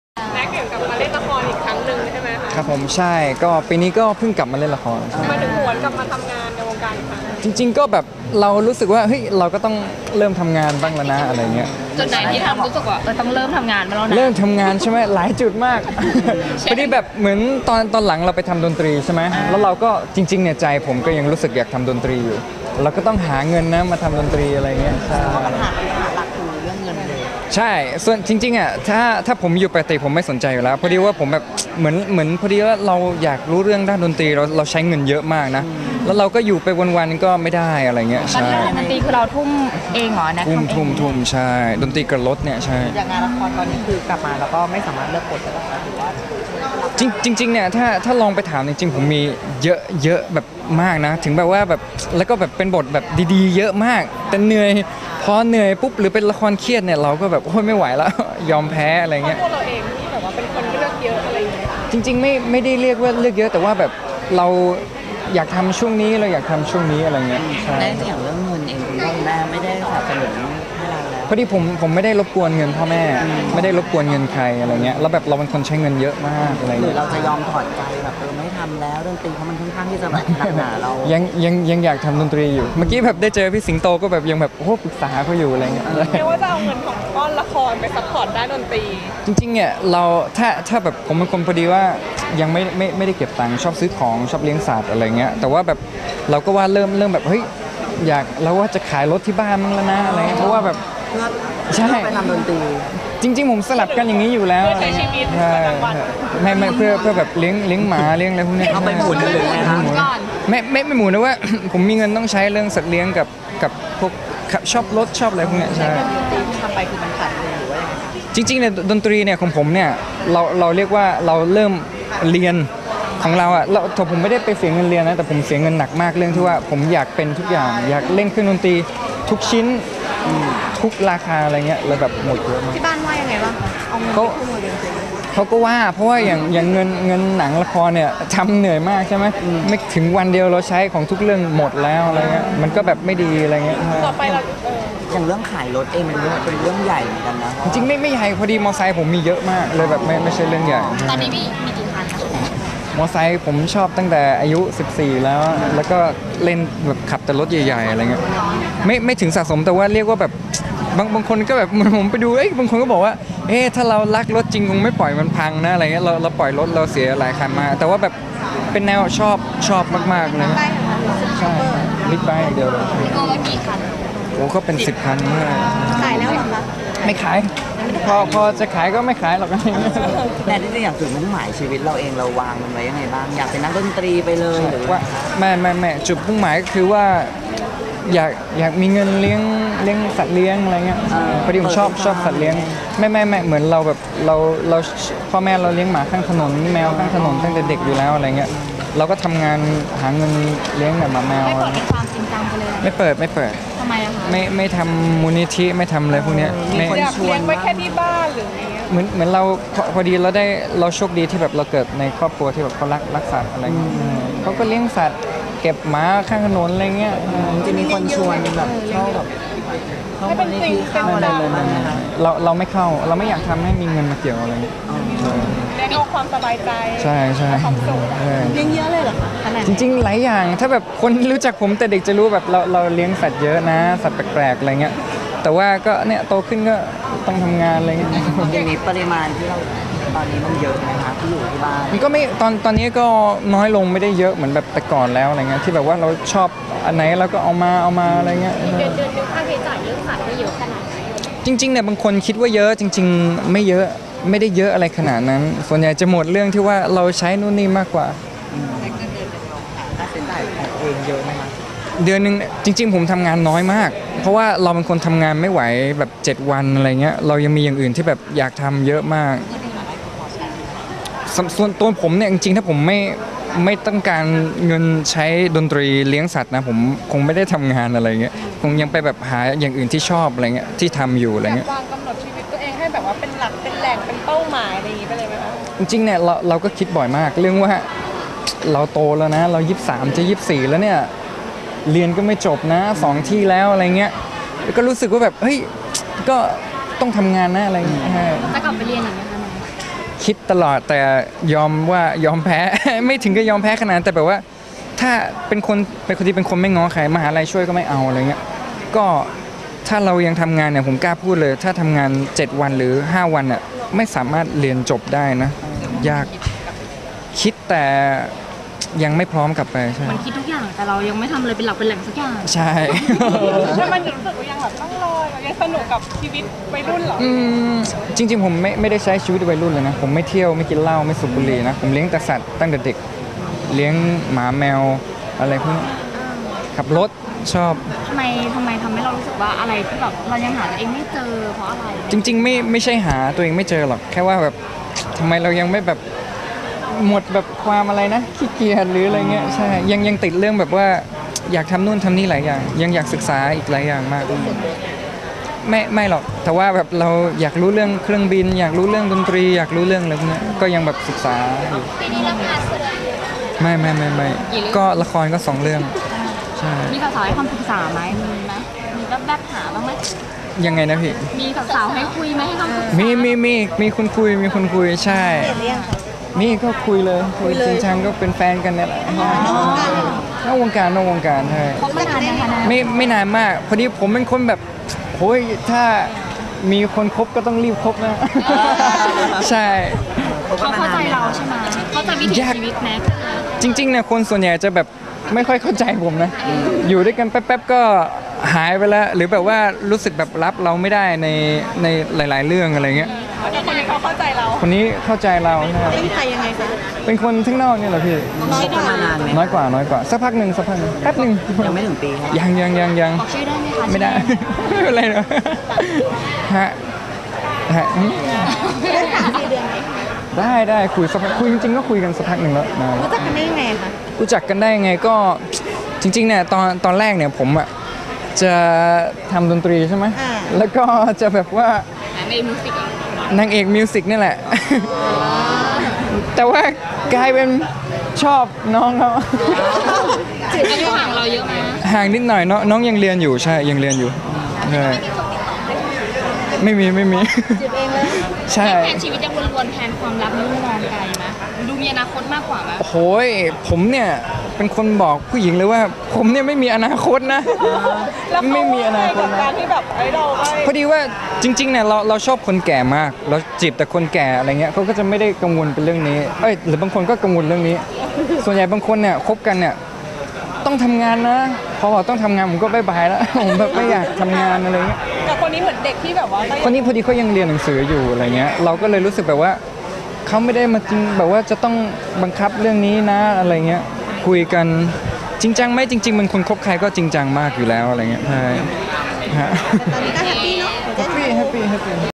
แน็คชาลีรับหันลุยงานวงการเหตุเรื่องเงินมีคิดขายมอเตอร์ไซต์อยากกลับไปเรียน
แน็ค ชาลี หวนรับงานละคร2เรื่อง รับเหตุผลหลักเพราะเรื่องเงิน โอดหายไปทำเพลงต้องใช้เงินเยอะ แจงคิดอยากกลับไปเรียนแต่ไม่พร้อม เปรยส่วนตัวหมดเงินไปกับเลี้ยงสัตว์และทำเพลง แย้มคิดขายมอเตอร์ไซค์ ฟุ้งรักสาวนอกวงการรื่น. อ่านต่อได้ที่